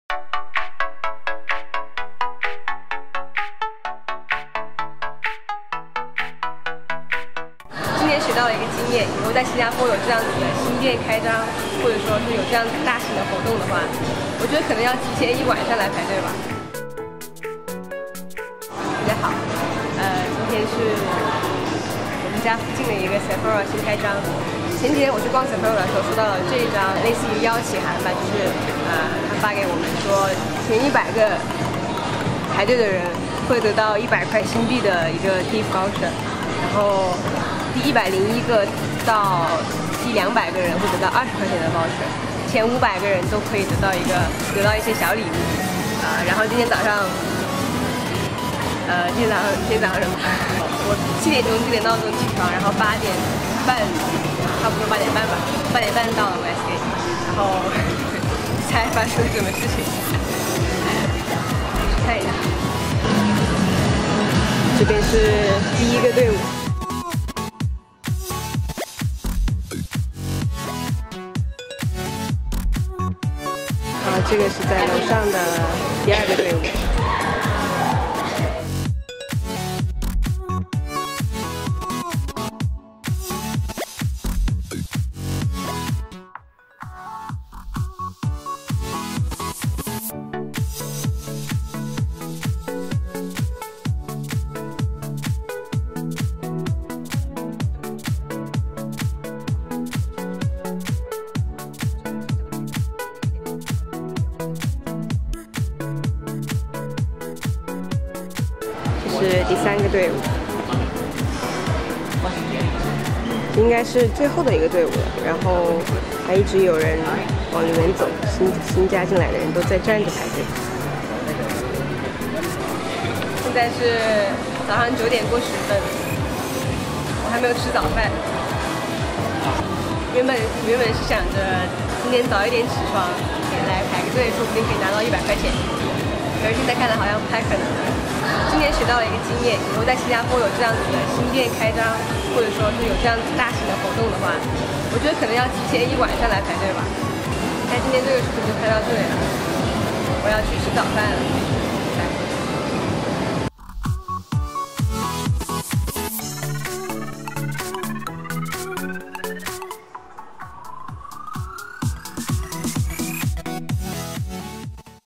SEPHORA 新店开业排队记
Hello 大家好~
我是在新加坡留学的中国学生
我的频道会经常更新分享我的留学生活

排队在新加坡是一种文化
今天我去了 SEPHORA 新店开张，妥妥的见识了排队大军
一起来看看吧~

p.s. 这家店是从JEM（一家商场）搬迁到了Westgate（另一家商场），2018年2月1日新店开业，新店地址是 Westgate Shopping Mall, 3 Gateway Drive, #01-23/24, Singapore (608532)，营业时间是10am - 10pm。

Other Keywords: 南洋理工大学, 新加坡大学,NTU, Nanyang Technological University